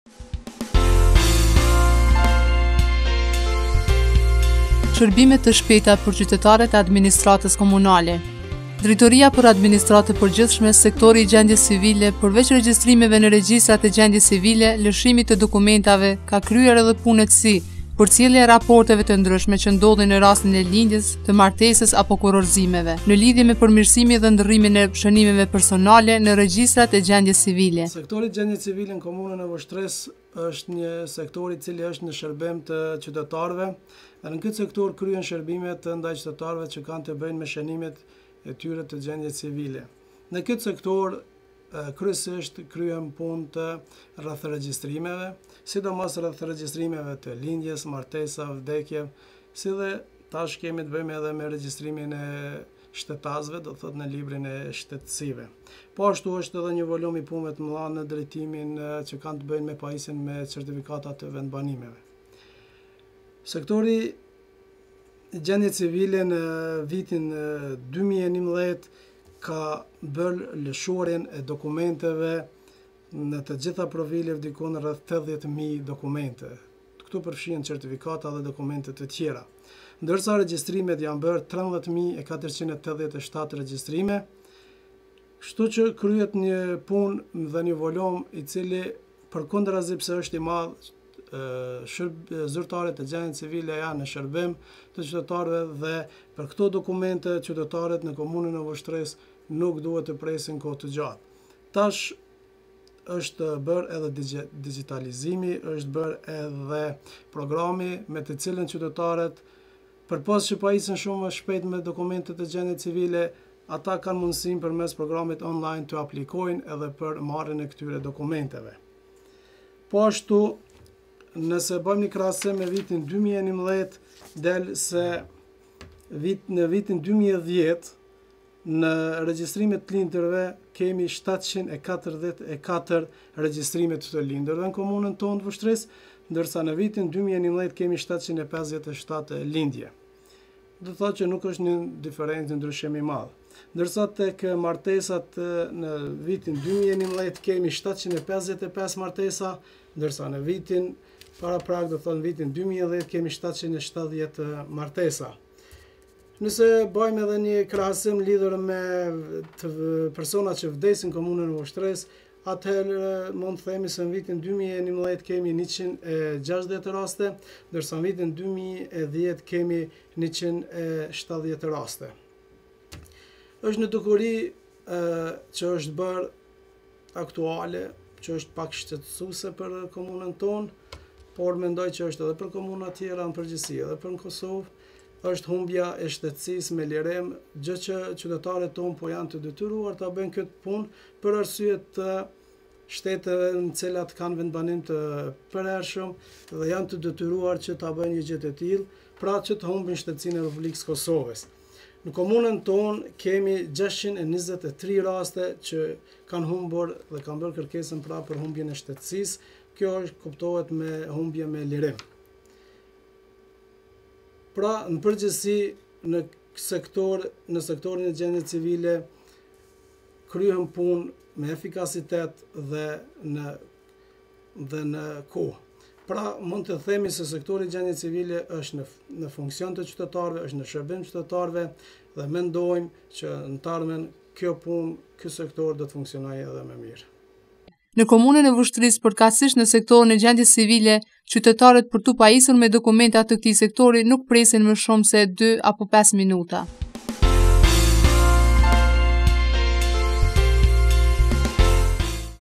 Administrata komunale qytetarëve u ofron shërbime të shpejta
Në vitin  2011 janë lëshuar 70 999 certifikata të ndryshme, nga sektori i gjendjes civile